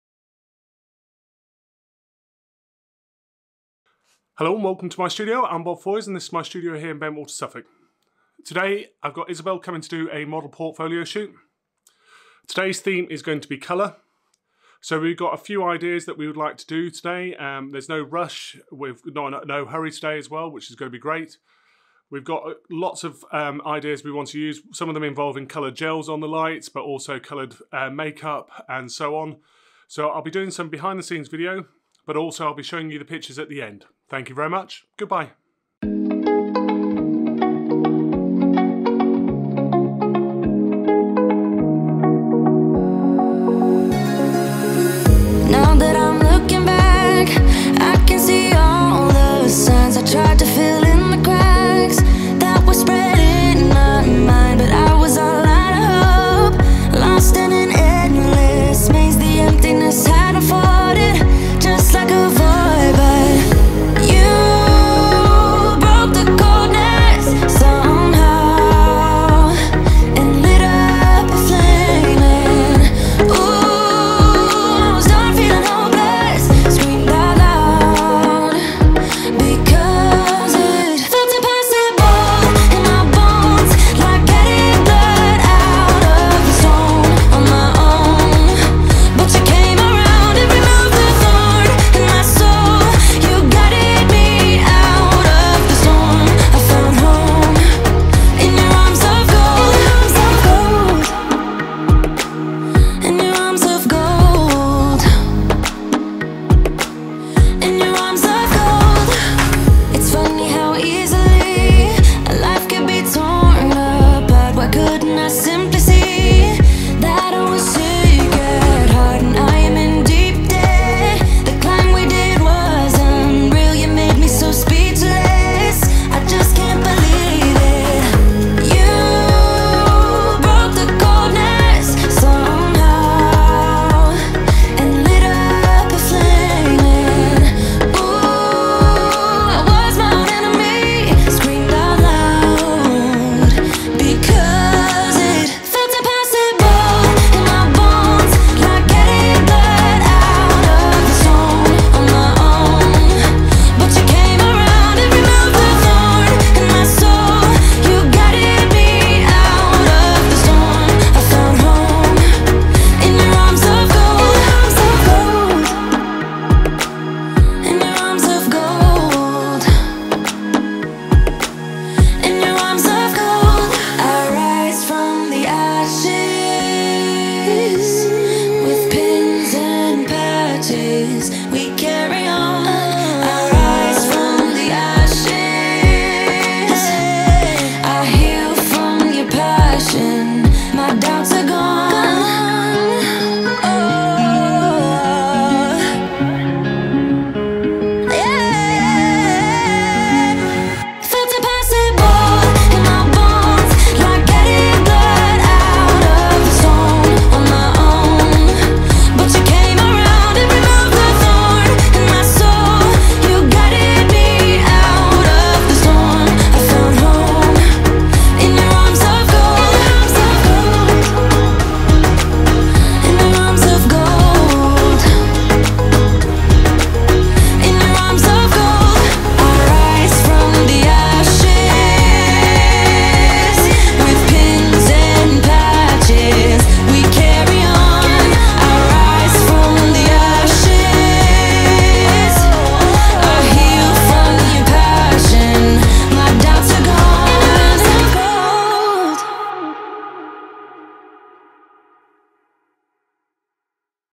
Behind the scenes - Colour-themed portrait shoot with Isabelle
Quick behind the scenes of a colour-themed portrait shoot with Isabelle